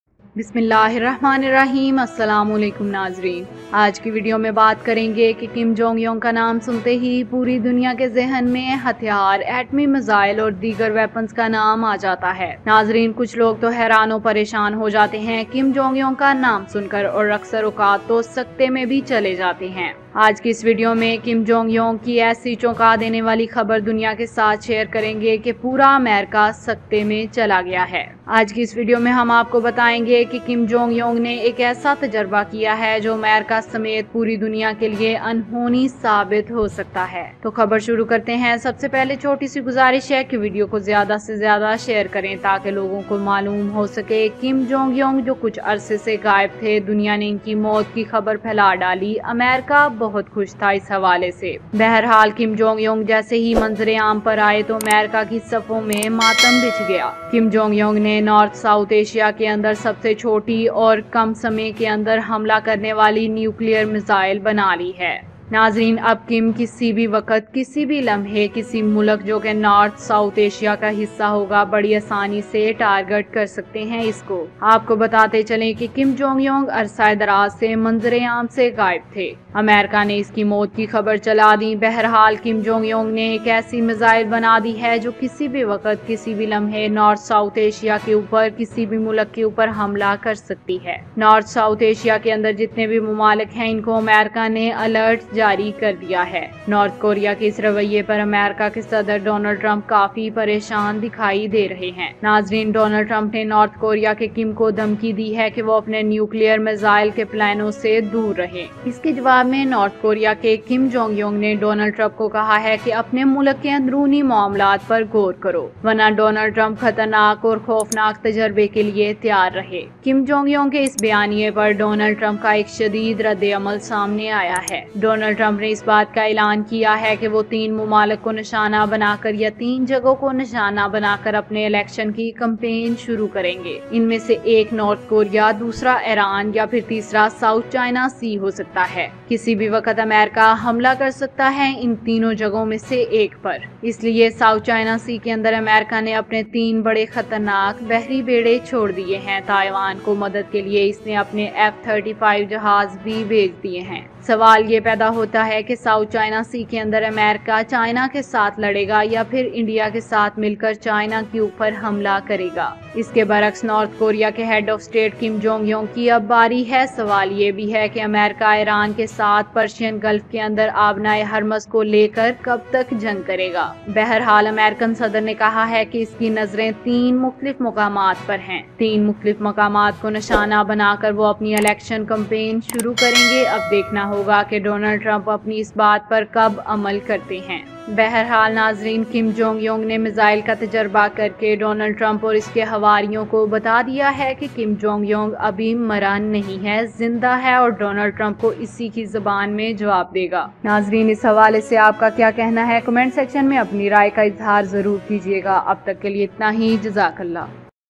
North Korea Threatens Its Nukes Could Destroy United States | Kim Jong Un 'New Strategic Weapon'
Hello Friends In Today's Video I Will Tell You About North Korea Threatens Its Nukes Could Destroy United States | Kim Jong Un Warns Of North Korea's 'New Strategic Weapon' | In Hindi Urdu

|Saudi Arabia Riyal Exchange Rate Today| |28 June 2020| |Today Riyal Exchange Rate All Countries| |Arab News TV|